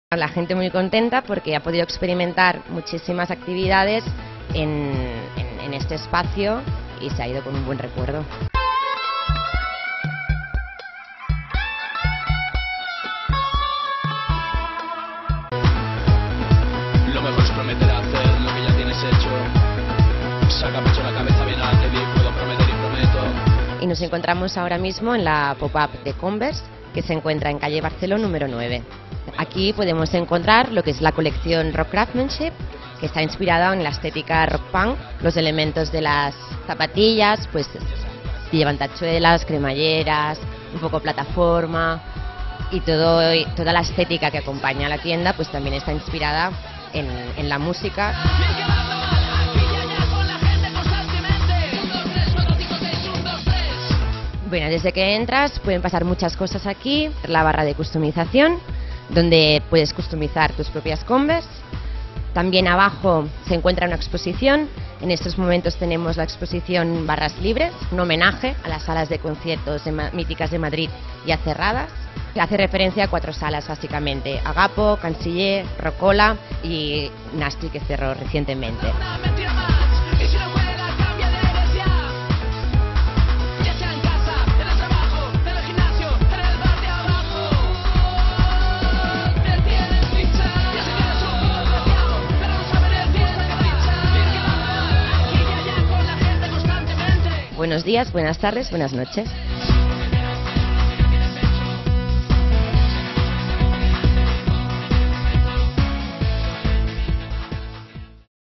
Garito de converse - Tendencias.tv #587
El pasado mes de octubre pudimos disfrutar de la experiencia Pop up de Converse en Madrid. Allí, se abrieron las puertas de este local que durante un mes llenó la agenda madrileña con conciertos, desfiles, talleres y exposiciones. No nos quisimos perder la oportunidad de conocer este Garito antes de que sus puertas se cerraran para siempre. Para siempre, para siempre, no... porque amenazan con volver a abrir pero en una ciudad distinta. ¿Barcelona? ¡Lo veremos! El Garito de Converse lleno de ideas, ¿entramos?